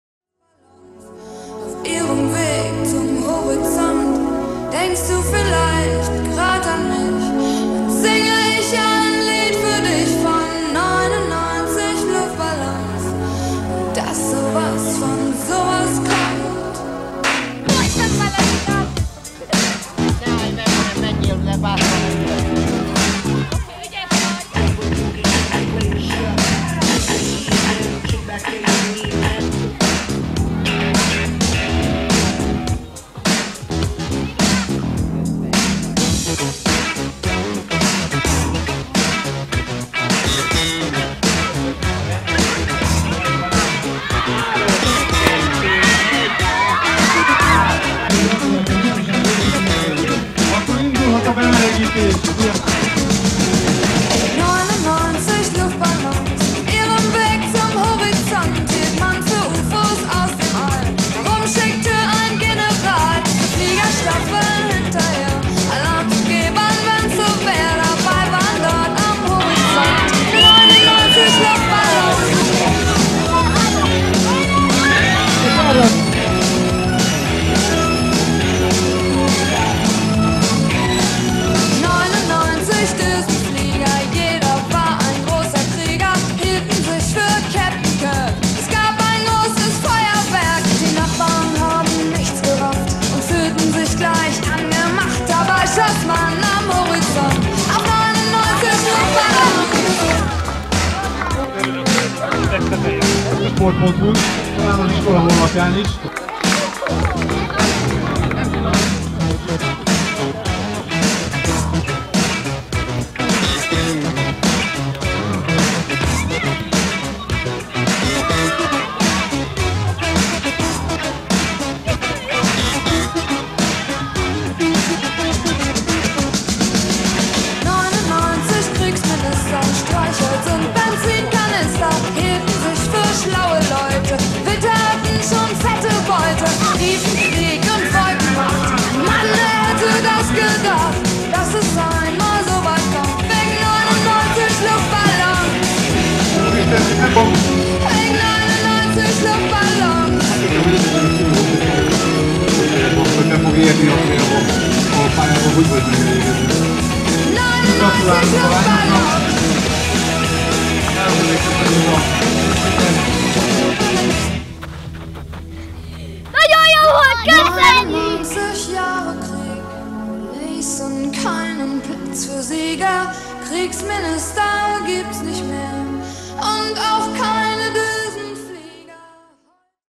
Csömöri szabadtéri sportközpont átadása
Szabadtéri sportközponttal bővült a Csömöri Sportcsarnok 2014-ben, ennek köszönhetően még több helyi sportolási lehetőségből választhatunk. A sportcentrum létesítményén belül 308 székes lelátóval rendelkező nagypályás élőfüves futballpálya, hatsávos, 400 méteres salakos futópálya, 2 távolugró gödör, magasugró hely, súlylökő pálya, kondi park és 2 teniszpálya épült.

A futópálya ünnepnapok kivételével minden nap napkeltétől napnyugtáig ingyenesen használható!

A sportcentrum átadása számos gyereket, felnőttet megmozgatott, nézzék meg összefoglalónkban!